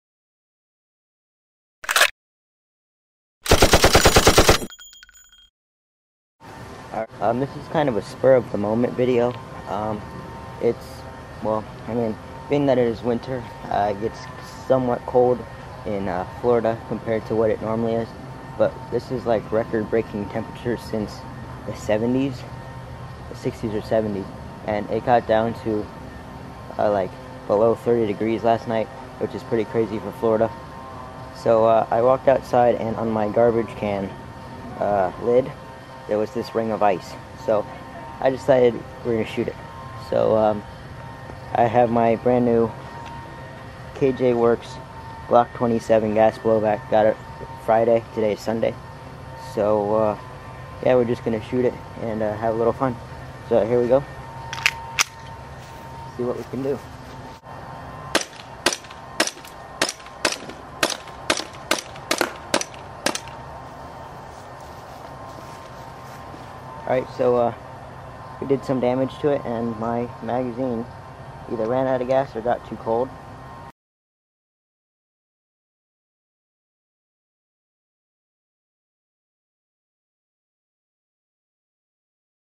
Shooting 1/2" Thick Ice with My KJW Glock 27 GBB
Another Aderonn AIrsoft Review. This was a spur of the moment video. It has been really cold this week and i found this ice on my trash can so i decided to shoot it for fun. At this temperature, i was experiencing problems with my gun and the pressure of the gas. It was almost to cold to shoot at all. Enjoy